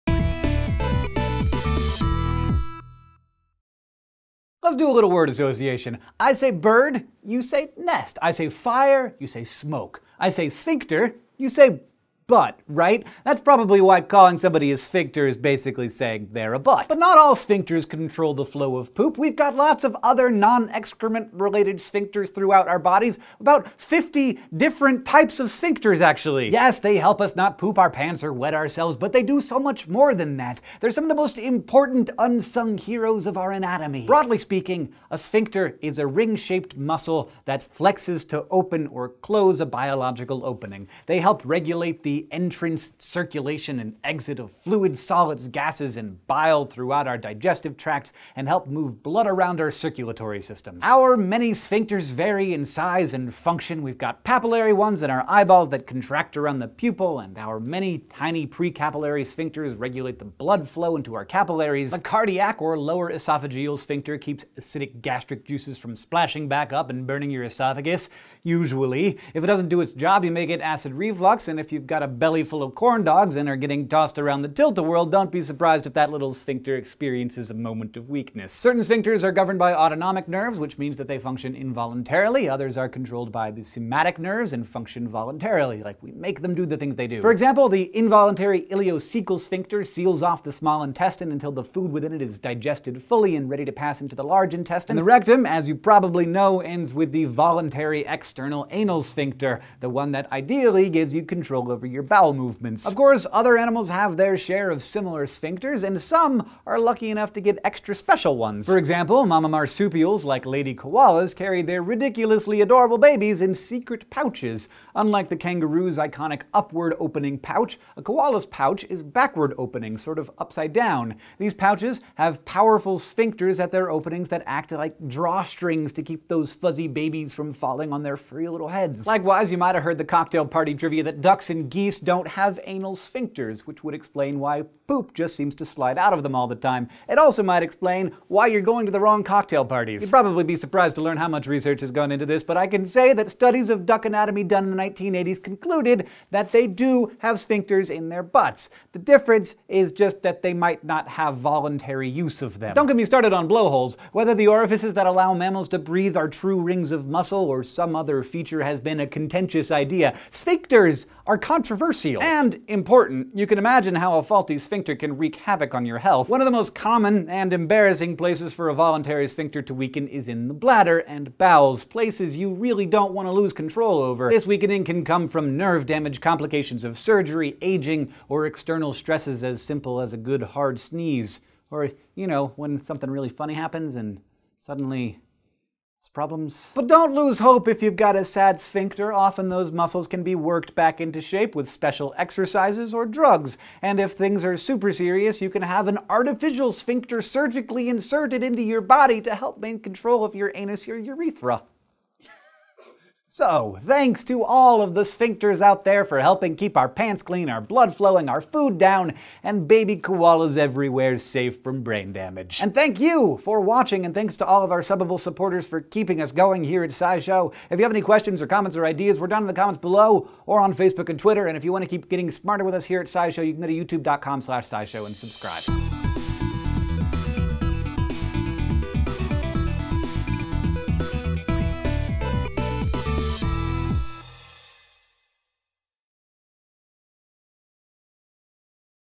Sphincters - The Fascinating Truth
Sphincters -- they're not just for butts! Hank explains the fascinating truth about these magic rings of muscle, where they appear in the human body and the pretty fantastic functions they perform in the animal kingdom.

Traci Adams to Gary Hughes -

I adore for you bringing out the nerdfighter in me.

Sources:
SOURCES